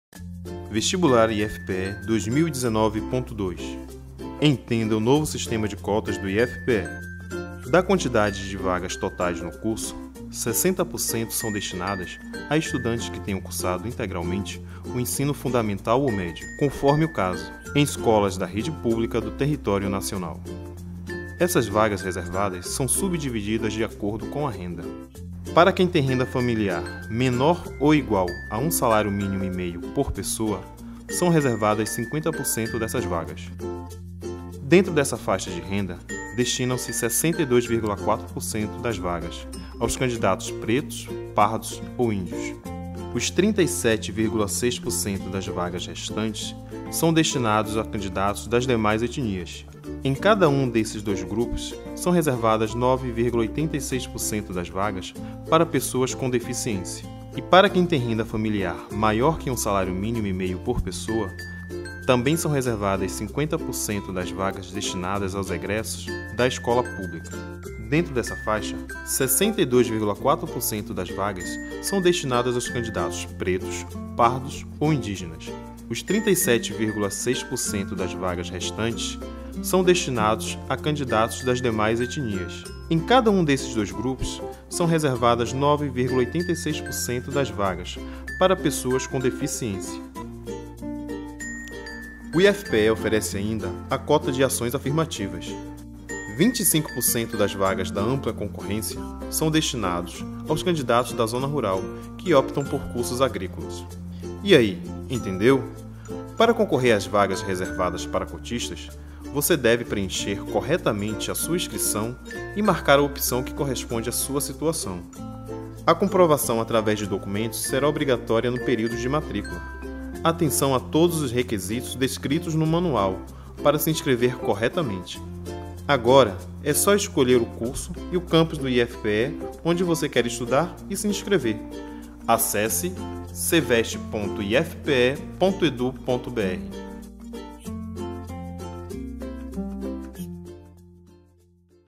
SIstema de Cotas Vestibular IFPE
Entenda como funciona o sistema de cotas. A partir da edição 2019.2 o percentual de vagas destinadas a estudantes oriundos da rede público de ensino passa a ser de 60%.